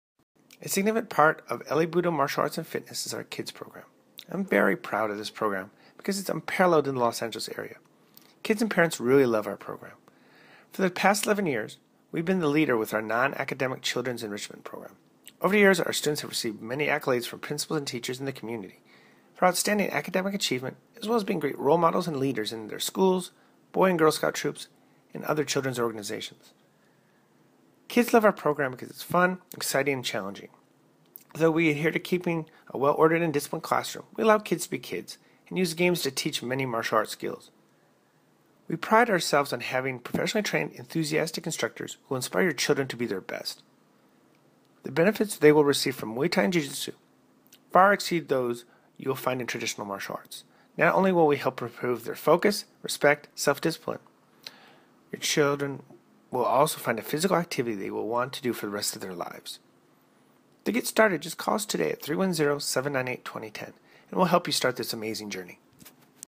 Kids Mixed Martial Arts Program in Manhattan Beach | Special Trial Program
Kids Mixed Martial Arts Program in Manhattan Beach L.A. Budo Martial Arts promotion video of their martial art program for Kids.